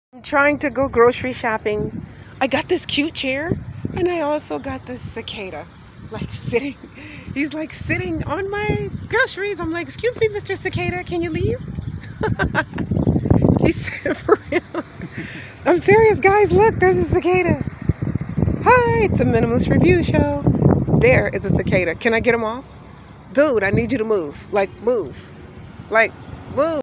Touching a cicada bugs wings!
okay this cicada bug landed on my things and so I pet its wings to try to get him to leave he did not want to leave me he was beautiful gold brown and dark fall summer colors he eventually flew away but he was so beautiful!! nature is amazing.